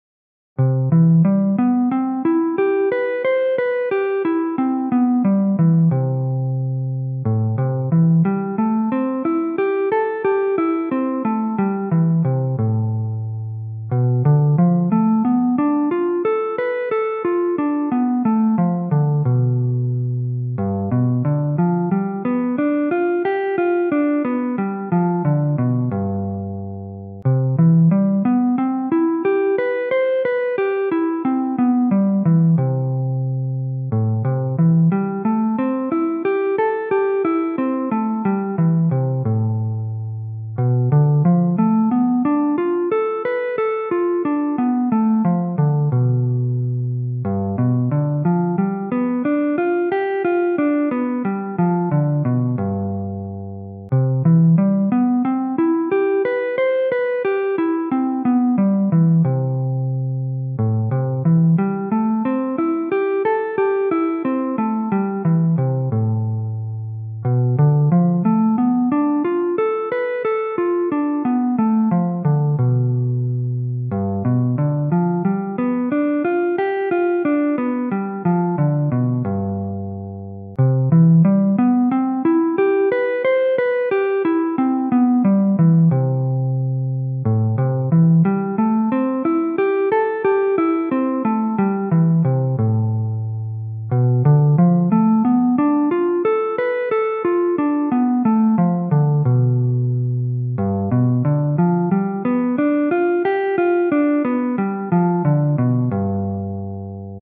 7th Chord Arpeggios | Progression in C Major (Cmaj7 - Am7 - Bm7b5 - G7)
A chord progression in C major on guitar using arpeggios. It isn't a super common progression, but it is useful for practicing since it includes all four different types of diatonic seventh chords found in a major scale. 1/8 notes at 90 BPM.